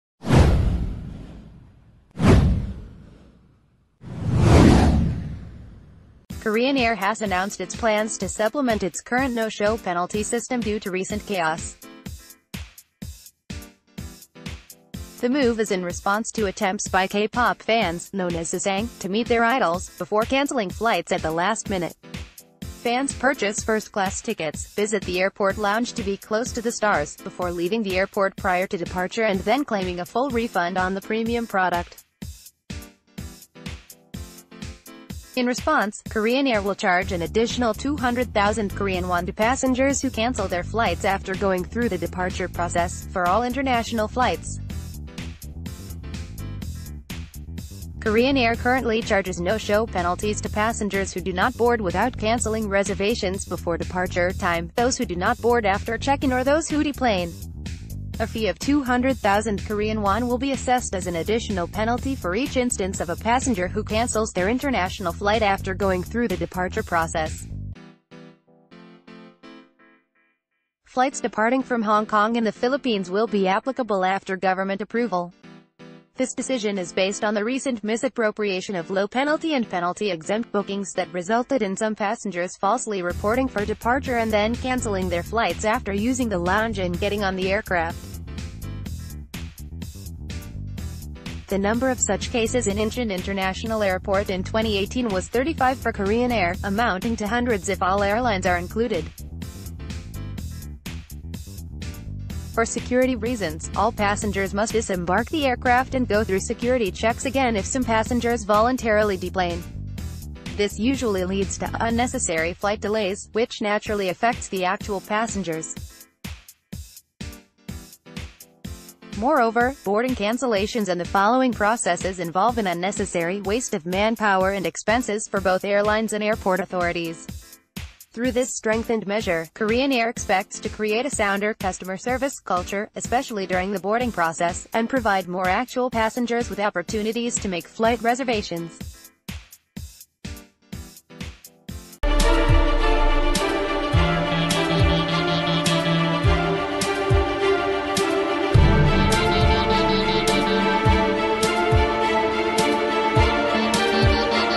K-pop fans force Korean Air to up no-show fees
Korean Air has announced its plans to supplement its current no-show penalty system due to recent chaos. The move is in response to attempts by K-pop fans, known as Sasaeng, to meet their idols, before cancelling flights at the last minute. Fans purchase first class tickets, visit the airport lounge to be close to the stars, before leaving the airport prior to departure and then claiming a full refund on the premium product. In response, Korean Air will charge an additional KRW200,000 to passeng...